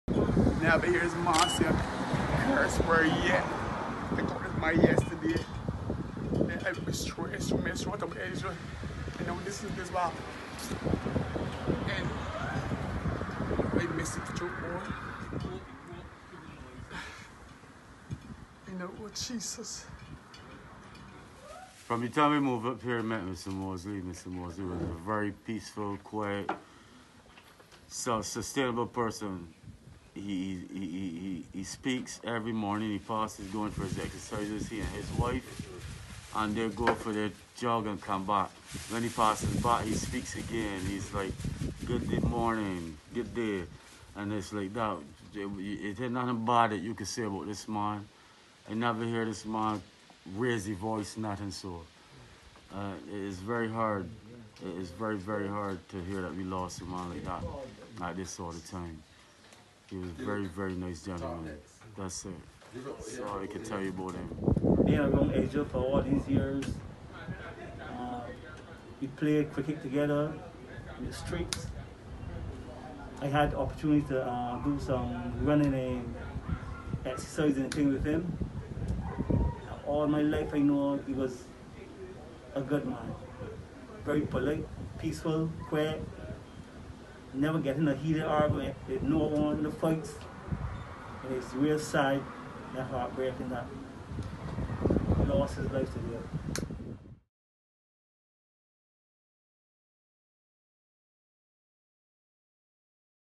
Nation Update: Remembering Ezra Moseley
Former Barbados and West Indies fast bowler Ezra Moseley died tragically in a road accident here Saturday morning.

Reports indicated that Moseley, who celebrated his 63rd birthday last month, was riding a bicycle along the ABC Highway when he was in a collision with an SUV driven at the Balls, Christ Church junction.

Nation News spoke to his cousin Owen Moseley and other members of the community at Waldron, Christ Church, who were saddened by the passing.

(Video by Jameel Springer)